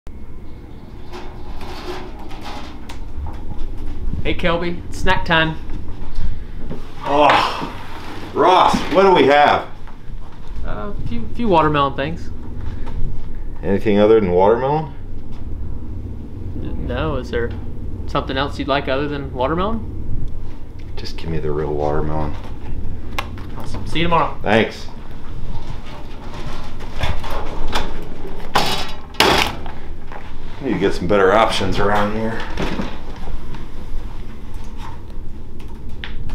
Snack Time with Ross
Snack time looks a little different with Ross Chastain in the office.